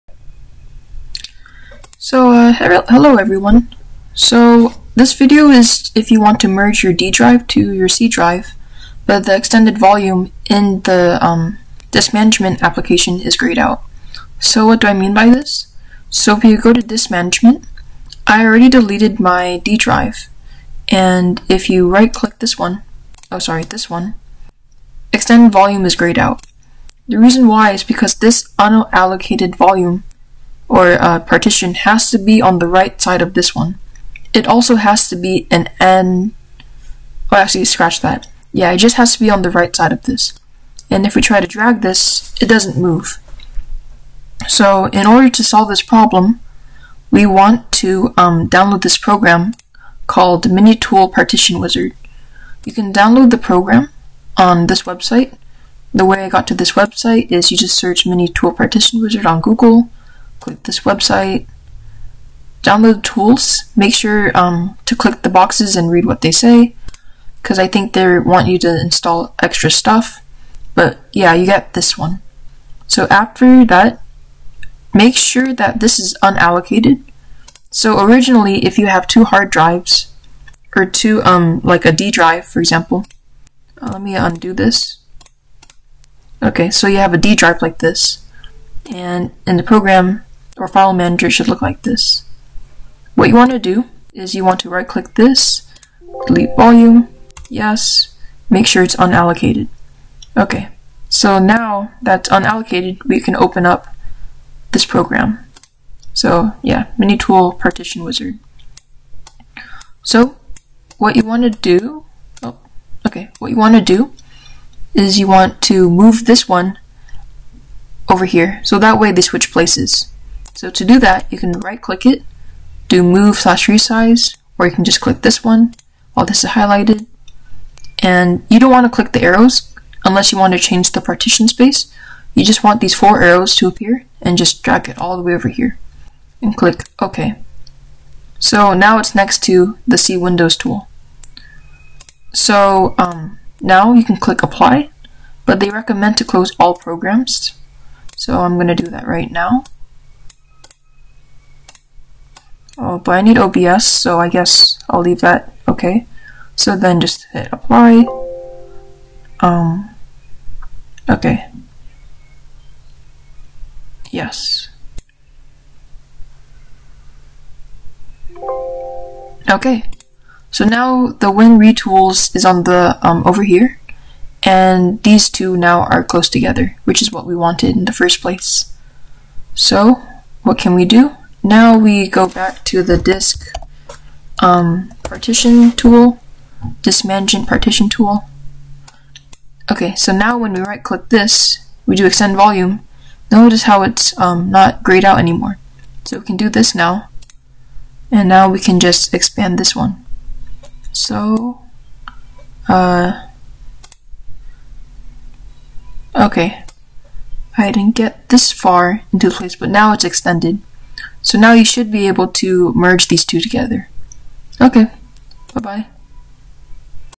How to Merge Partition Drives When Extend Volume is Greyed Out In Disk Management - External App
***Skip to 0:43 for solution***
WARNING!!: People are warning in the comments that this app is really sketchy and might have a virus. I am glad people are bringing this up. From here on out, please proceed with caution.

How to merge D Drive with C Drive when "Extend Volume" is greyed out in the disk management application tool in Windows 10.

This requires a certain app called MiniTool Partition Wizard

In my personal case, I have an MSI Ps63 Modern with 512 GB of SSD. When I purchased the laptop, the C: drive and the D: drive are separate. When combining them they are the ~512GB of SSD the laptop is advertised with.

Problem:
When going to Disk Management - Right-Click the C: drive - "Extend Volume" is greyed out

Solution:
We have to make the D: drive into an unallocated volume. Then we need to move the allocated volume to the right side of the C: drive

1. In Disk Management - Right-click D: drive - click "Delete Volume"
The volume should then be a blank volume

2. Download Mini Partition Wizard - click on "WinRE Tool" to highlight it - click "Resize/Move Partition" - drag the partition with the 4 arrow cursor all the way to the right - click apply

3. Go back to Disk Management - Right click C: drive and do "Extend Volume".

A pop up window will appear. Say continue - continue again after another pop up window appears

You can also check out this link for how to do Step 3

4. The C: drive should now have more memory